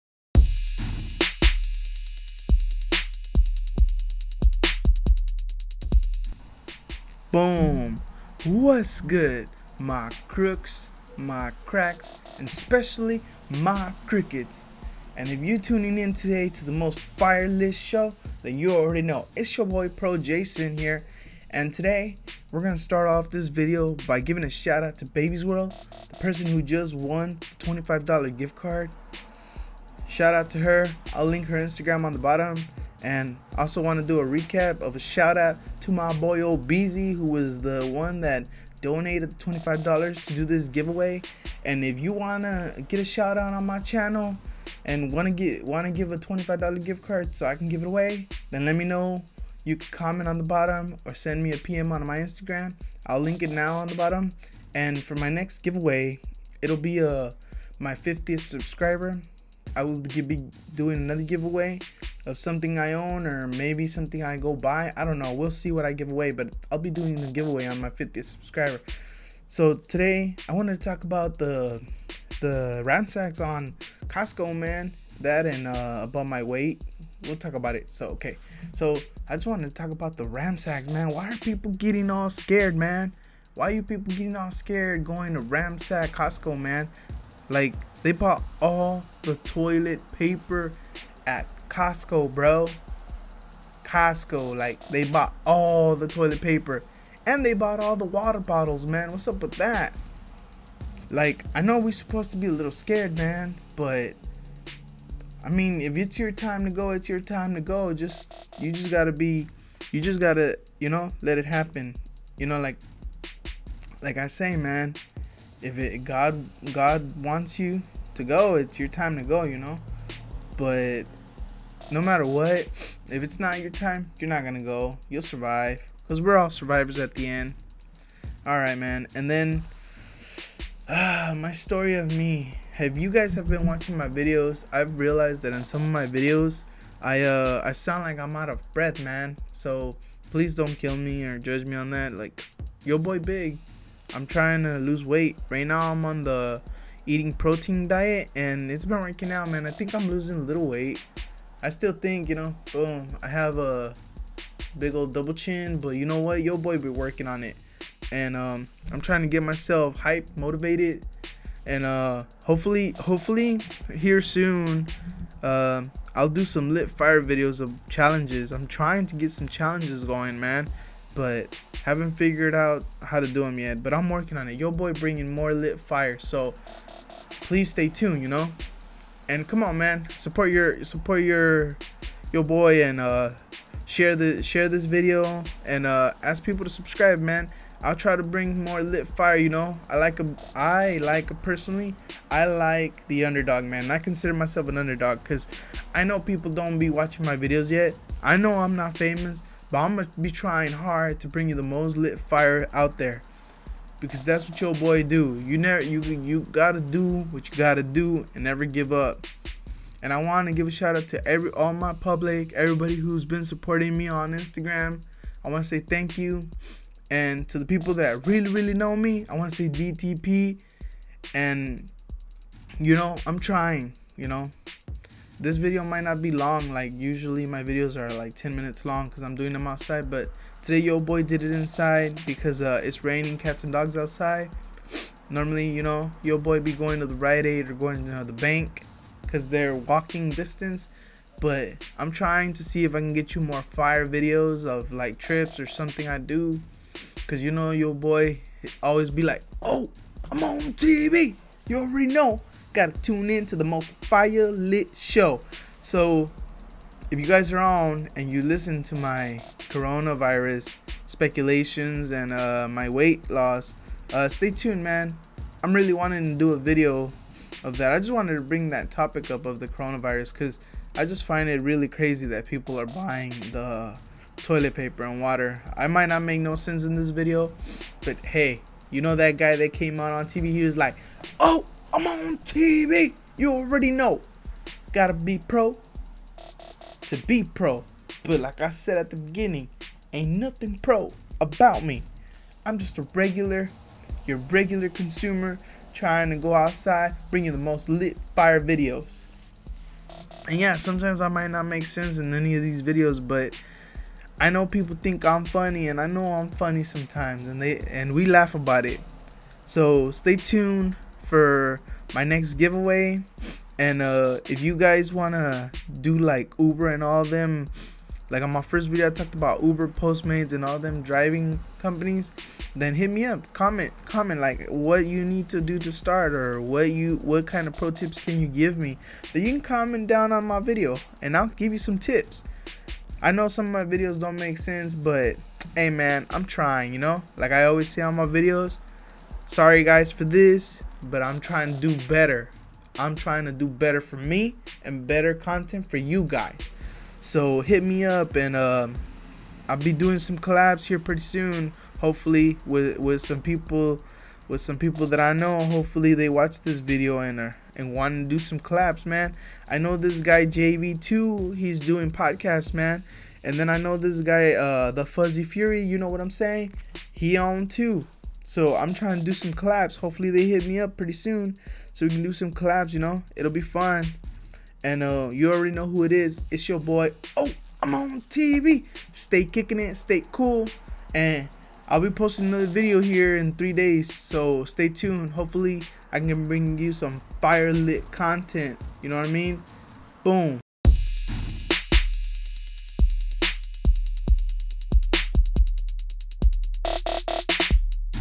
Randomness by your's truly Vlog#7
I give shout outs and talk about my weight and corona virus